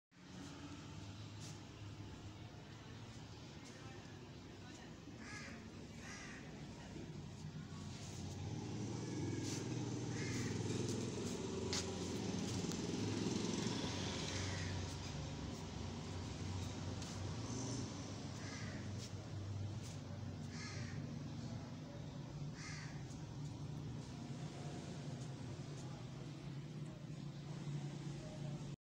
us 125 united 2021 model my new bike Alhamduillah
2021 model us united 125 inshallah both sari baty bataou ga is bike k bary m phely ma khud check kr lu